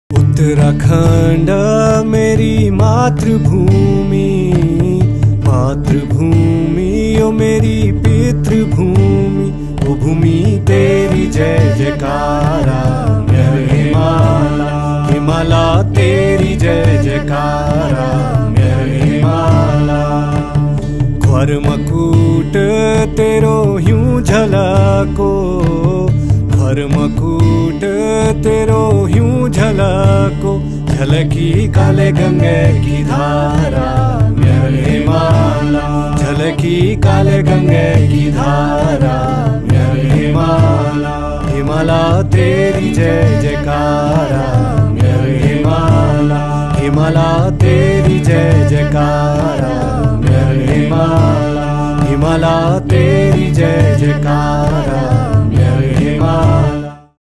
Himalaya Diwas...
"The Himalaya Diwas"  is celebrated every year in state of Uttarakhand after official declaration from the then Chief Minister of Uttarakhand  Sh. Harish Rawat in 2014.  This day is celebrated to raise awareness about the conservation of the rich flora and fauna of the state and propagate the message of conservation of the Himalayan ecosystems.